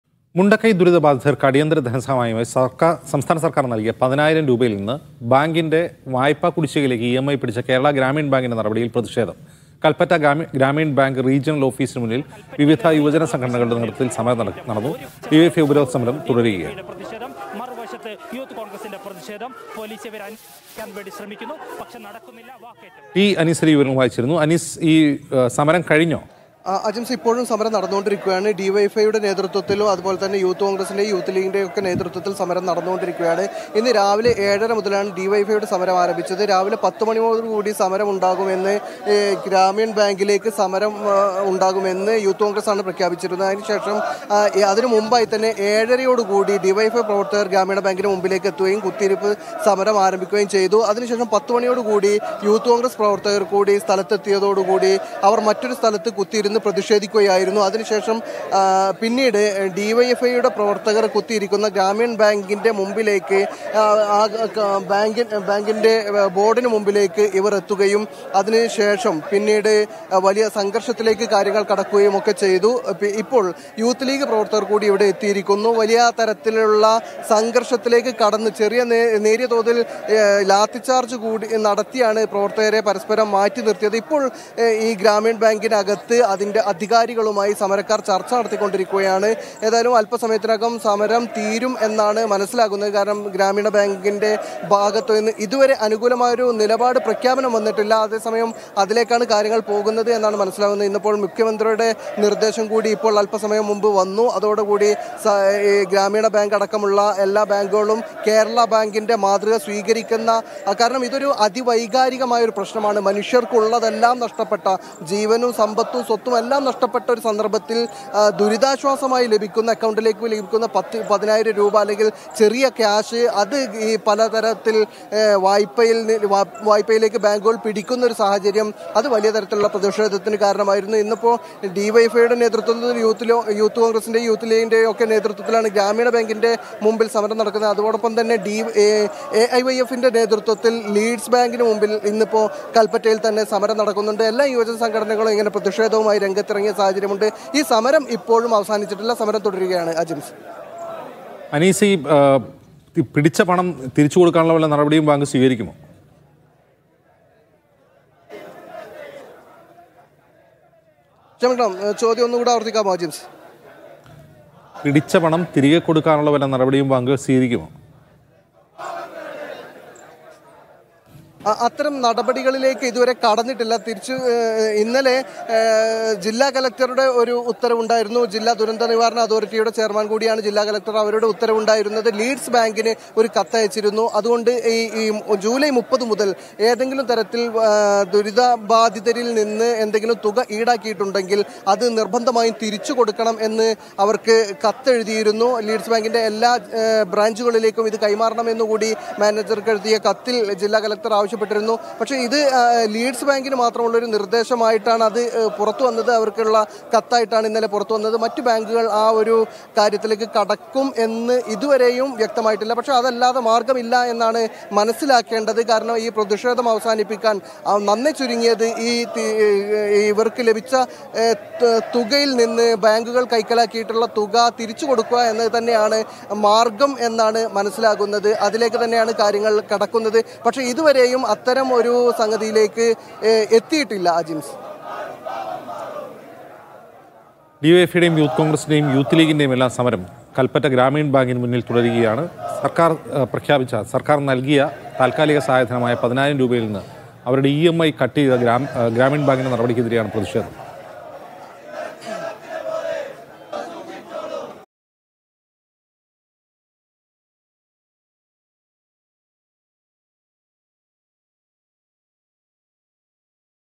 സഹായധനത്തിൽ നിന്ന് EMI; ഗ്രാമീൺ ബാങ്കിന് മുന്നിൽ പ്രതിഷേധം, പൊലീസുമായി ഉന്തും തള്ളും
സഹായധനത്തിൽ നിന്ന് EMI; ഗ്രാമീൺ ബാങ്കിന് മുന്നിൽ പ്രതിഷേധം, പൊലീസുമായി ഉന്തും തള്ളുംMalayalamNewsLive
Genre: News
Language: Malayalam